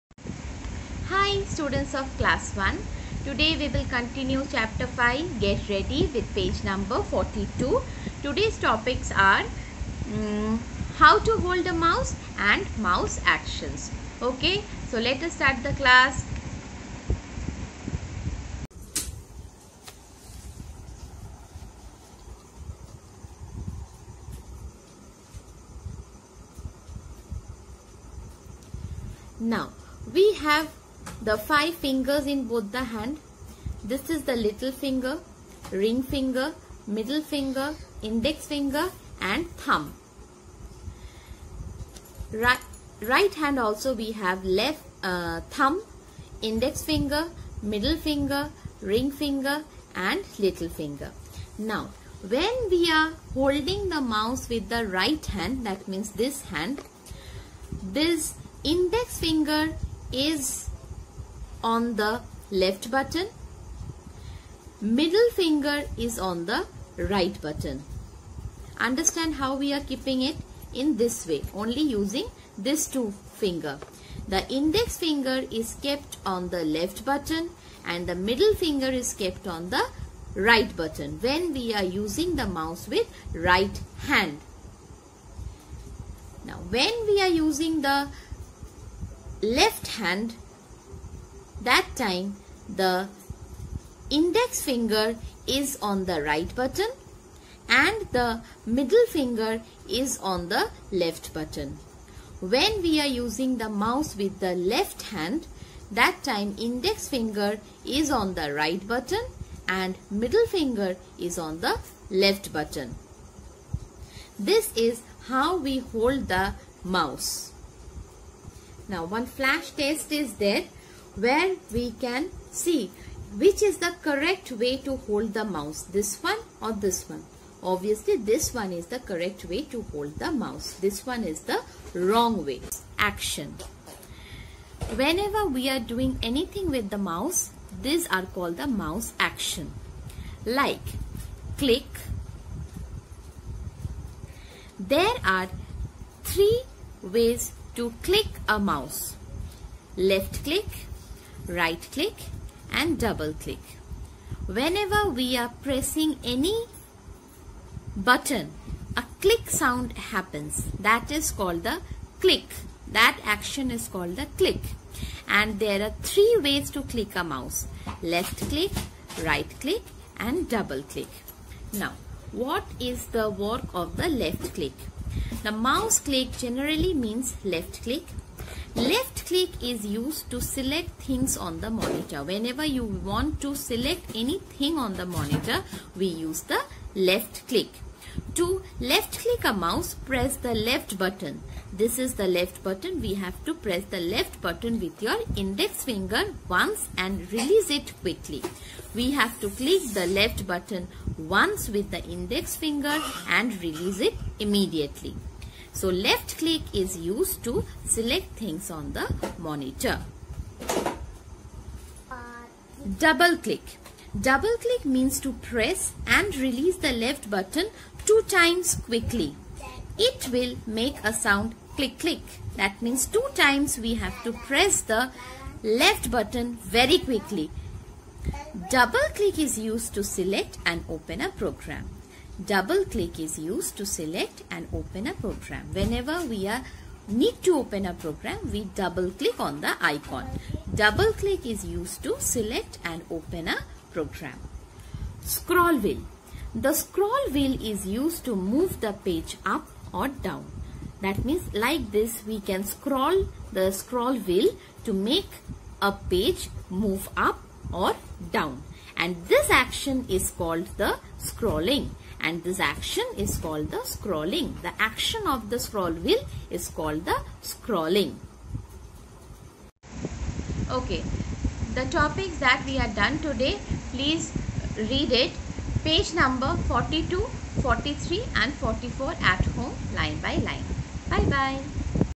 STD-1, COMPUTER_CH5_PART2
Cw- pg -42-45 explanation done. Topic- holding a mouse, mouse actions.
Hw- read pg- 40-45 line by line.